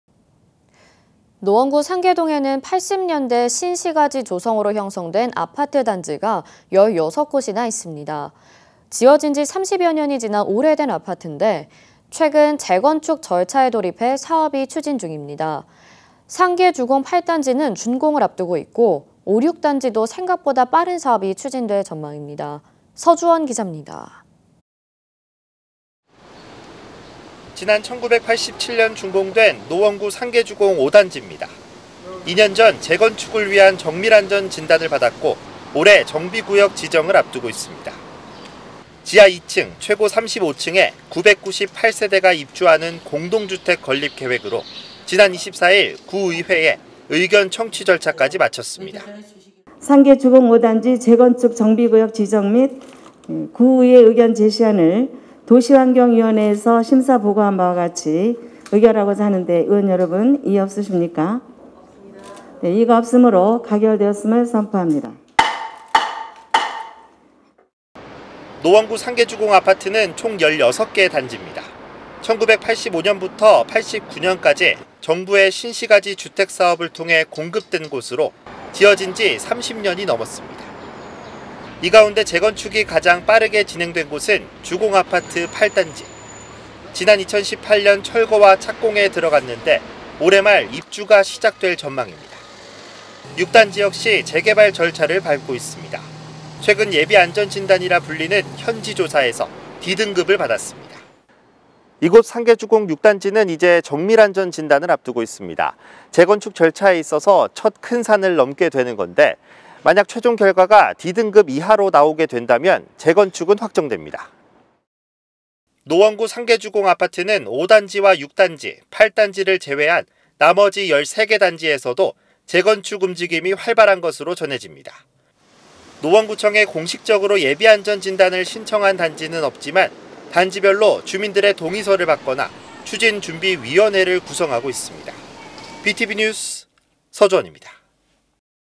노원구 상계주공 재건축..."3개 단지 사업 추진"_SK broadband 서울뉴스
[서울뉴스 서주헌기자]

[앵커멘트]
다음은 노원구 소식입니다.
아파트가 밀집한 노원구 상계동에는 80년대
신시가지 조성으로 형성된 아파트 단지가
16곳이나 있습니다.
지어진 지 30여 년이 지난 오래된 아파트인데
최근 재건축 절차에 돌입해 사업이 추진중입니다.
상계주공 8단지는 준공을 앞두고 있고
5, 6단지는 생각보다 빠른 사업이
추진될 전망입니다.
서주헌 기자입니다.

[기사내용]
지난 1987년 준공된
노원구 상계주공 5단지입니다.

2년 전 재건축을 위한 정밀안전진단을 받았고
올해 정비구역 지정을 앞두고 있습니다.

지하 2층 최고 35층에 998세대가 입주하는
공동주택 건립계획으로 지난 24일
구의회의 의견청취 절차까지 마쳤습니다.

최윤남ㅣ노원구의회 의장
(상계주공 5단지 재건축 정비구역 지정 및
구의회 의견 제시안을 도시환경위원회에서
심사 보고한 바와 같이 의결하고자 하는데
의원 여러분 이의가 없으십니까? (네. 없습니다.)
네. 이의가 없으므로 가결됐음을 선포합니다.)

노원구 상계주공 아파트는
총 16개 단지입니다.

1985년부터 89년까지
정부의 신시가지 주택 사업을 통해 공급된 곳으로
지어진 지 30년이 넘었습니다.

이 가운데 재건축이 가장 빠르게 진행된 곳은
주공 아파트 8단지.

지난 2018년 철거와 착공에 들어갔는데
올해 말 입주가 시작될 전망입니다.

6단지 역시 재개발 절차를 밟고 있습니다.

최근 예비안전진단이라 불리는
현지조사에서 D등급을 받았습니다.

(이곳 상계주공 6단지는 이제
정밀안전진단을 앞두고 있습니다.
재건축 절차에 있어서 첫 큰산을 넘게 되는 건데
만약 최종 결과가 D등급 이하로 나오게 된다면
재건축은 확정됩니다.)

노원구 상계 주공 아파트는
5단지와 6단지, 8단지를 제외한
나머지 13개 단지에서도 재건축 움직임이
활발한 것으로 전해집니다.

노원구청에 공식적으로
예비안전진단을 신청한 단지는 없지만
단지별로 주민들의 동의서를 받거나
추진 준비위원회를 구성하고 있습니다.

Btv뉴스 서주헌입니다.

(촬영/편집- 김환 기자)